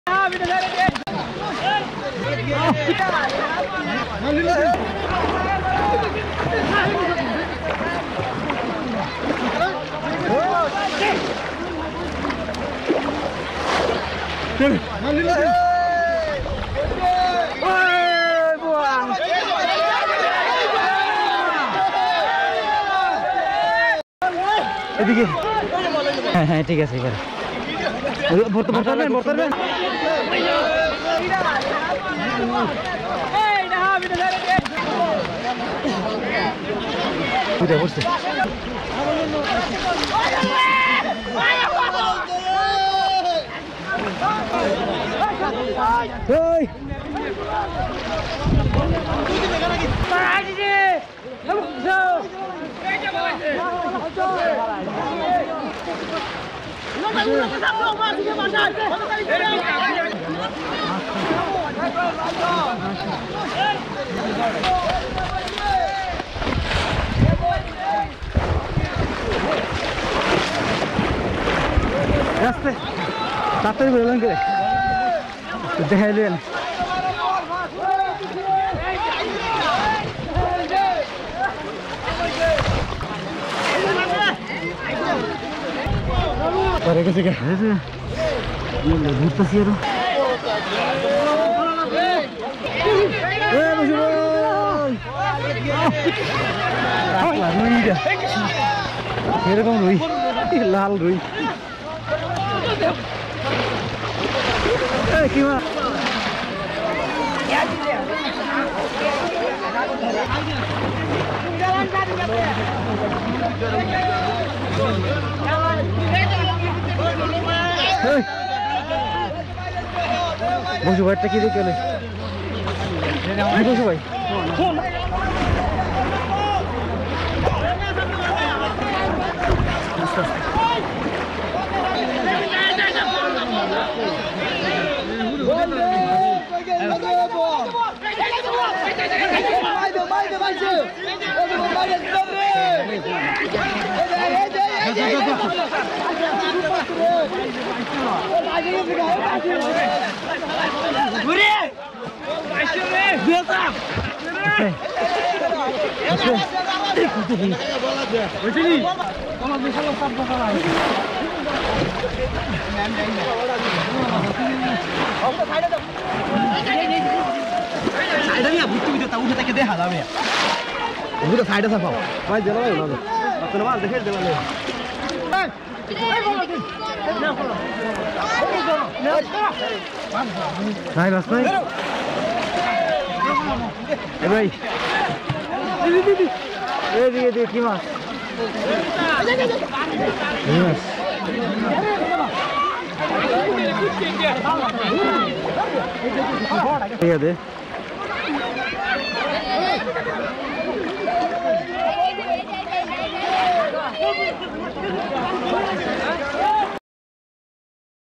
Village Small Pond In Amazing Big Sheat Fish Hunting.
Village fisherman best amazing big sheat fish hunting with bamboo polo.